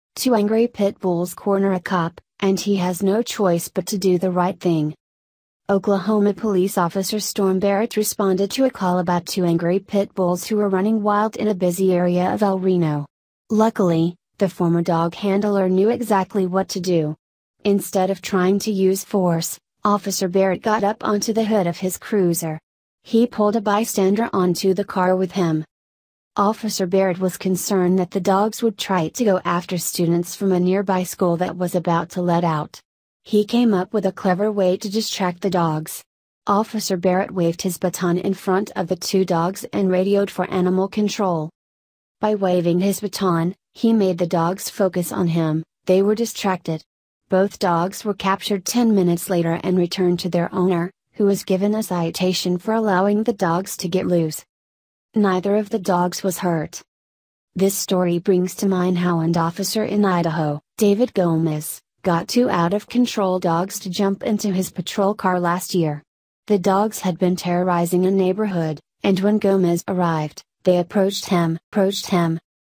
Two Angry Pit Bulls Corner A Cop – And He Has No Choice But To Do The Right Thing
Oklahoma police officer Storm Barrett responded to a call about two angry pit bulls who were running wild in a busy area of El Reno. Luckily, the former dog handler knew exactly what to do. Instead of trying to use force, Officer Barrett got up onto the hood of his cruiser. He pulled a bystander onto the car with him.

Officer Barrett was concerned that the dogs would try to go after students from a nearby school that was about to let out. He came up with a clever way to distract the dogs. Officer Barrett waved his baton in front of the two dogs and radioed for animal control.
By waving his baton, he made the dogs focus on him – they were distracted. Both dogs were captured ten minutes later and returned to their owner – who was given a citation for allowing the dogs to get loose.